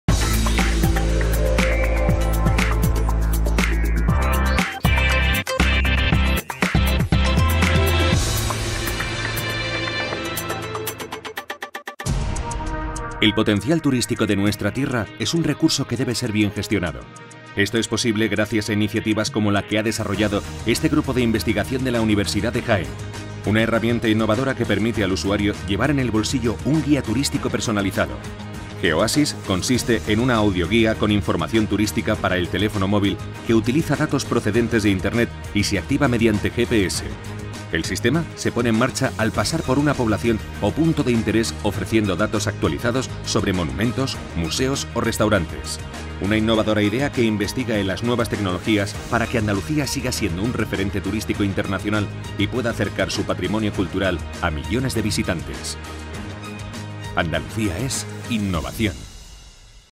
HDL GeOasis- Innovación
El potencial turístico de nuestra tierra es un recurso que debe ser bien gestionado. Esto es posible gracias a iniciativas como la que ha desarrollado este grupo de investigación de la Universidad de Jaén: una herramienta innovadora que permite al usuario llevar en el bolsillo un guía turístico personalizado.
GeOasis consiste en una audio-guía con información turística para el teléfono móvil que utiliza datos procedentes de Internet y se activa mediante GPS. El sistema se pone en marcha al pasar por una población o punto de interés ofreciendo datos actualizados sobre monumentos, museos o restaurantes.
Una innovadora idea que investiga en las nuevas tecnologías para que Andalucía siga siendo un referente turístico internacional y pueda acercar su patrimonio cultural a millones de visitantes.